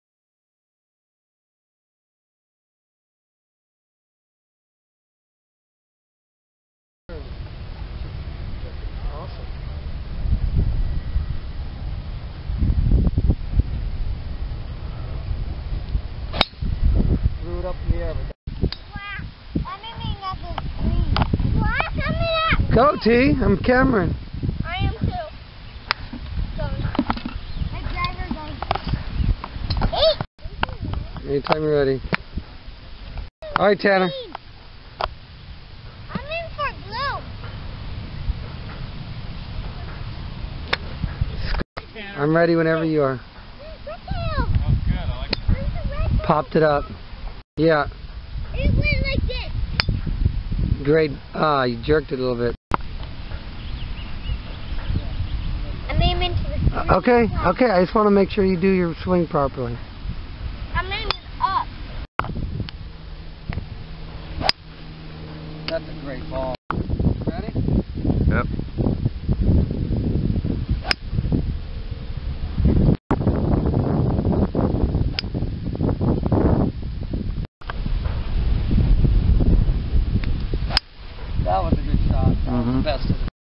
Golf swings from a range session in 2005
Tyler VanderVeer hitting balls at the range at Strawberry farms at 6 years old! Dylan and Tanner were 8 years old!